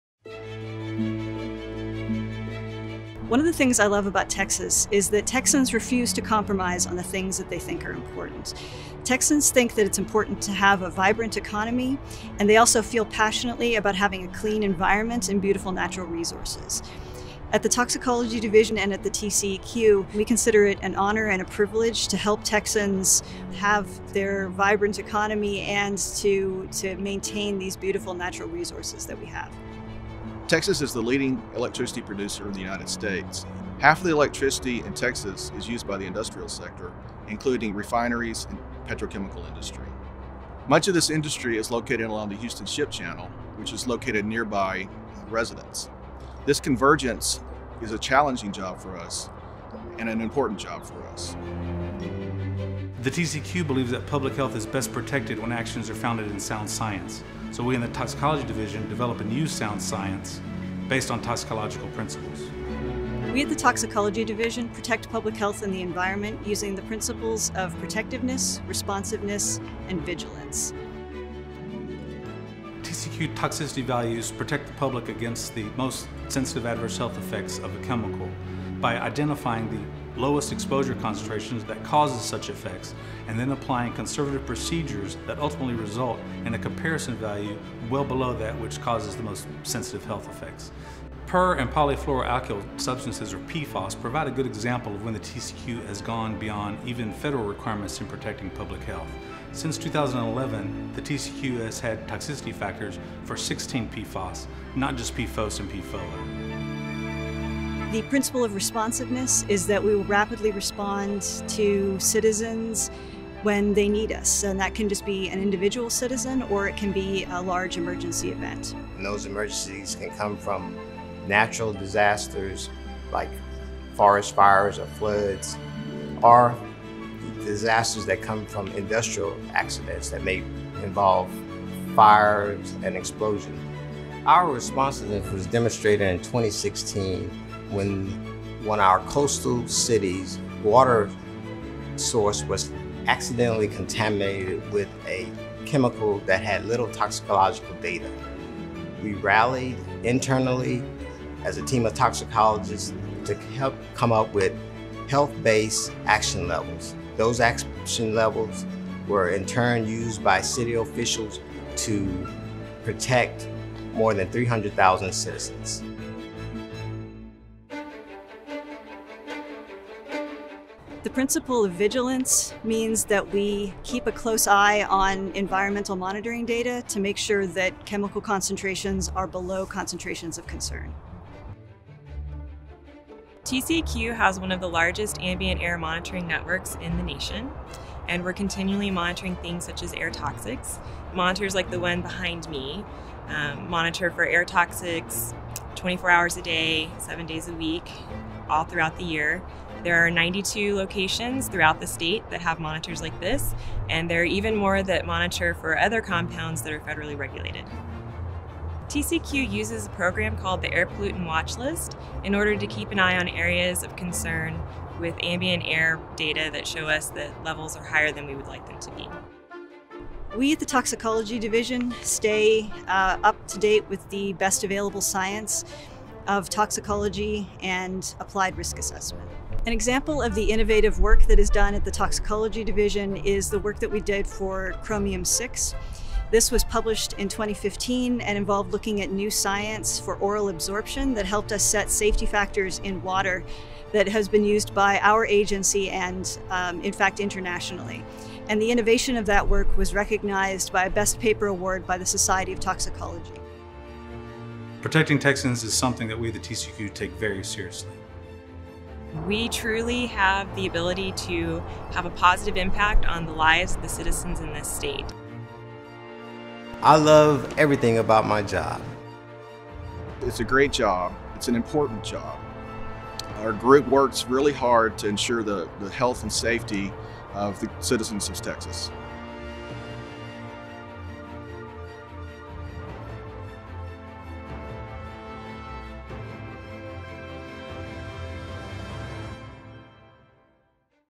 Texas Commission on Environmental Quality Toxicology, Risk Assessment, and Research Division
Protection Thru Sound Science.

Texas residents want a vibrant economy, and they also want their beautiful natural resources protected. Finding that balance is the task for the distinguished toxicologists at the T-C-E-Q. They're using sound science and the principles of protectiveness, responsiveness, and vigilance to apply toxicology and risk assessment methods to ensure the protection of people and natural resources in Texas.